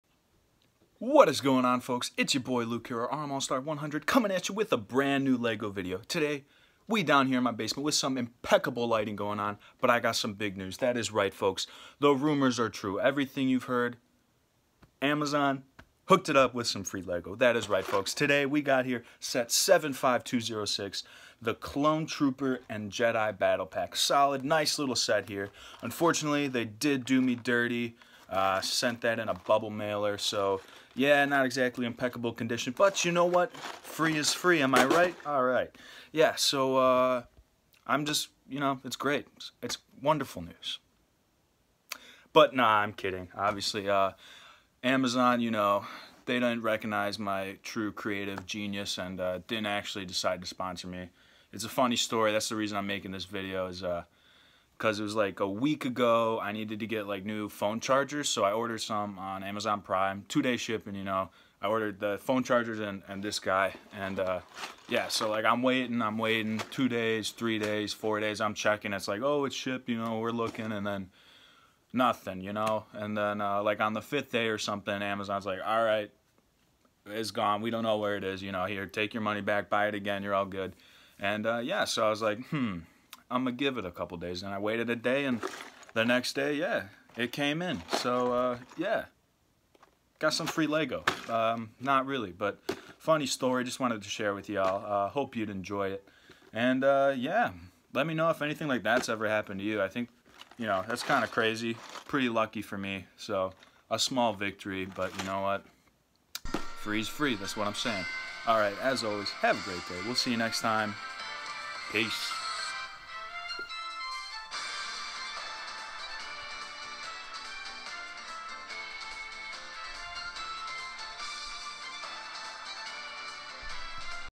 Amazon sent me FREE LEGO (Definitely Clickbait)
Today we taking a look at some FREE LEGO that amazon sent me for FREEEEE. But yeah its definitely clickbait. Enjoy this fun quick video!